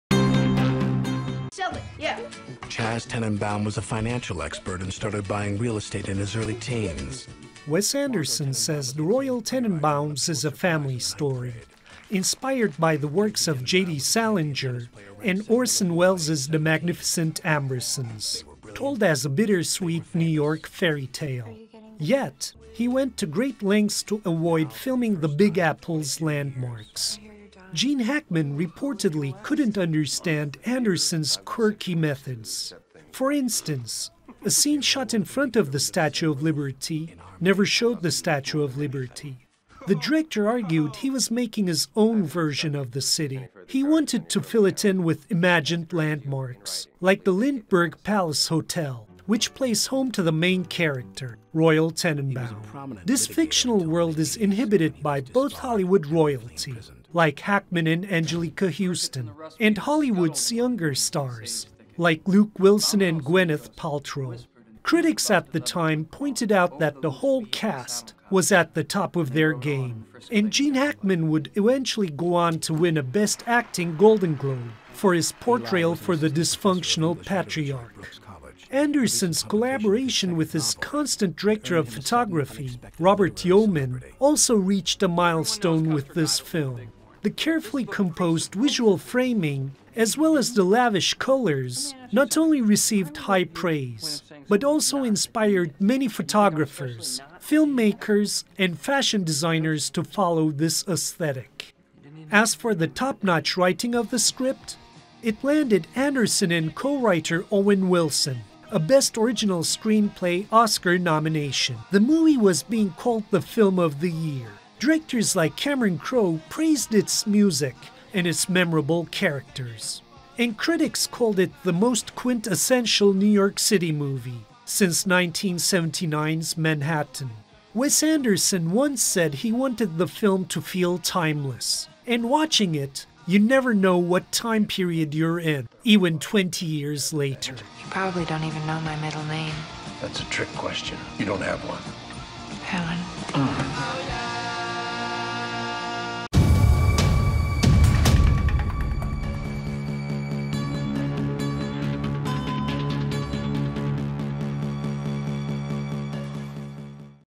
Movie Almanac: The Royal Tenenbaums
The Royal Tenenbaums cemented Wes Anderson's reputation as an influential director. We take a look at the movie 20 years after its release.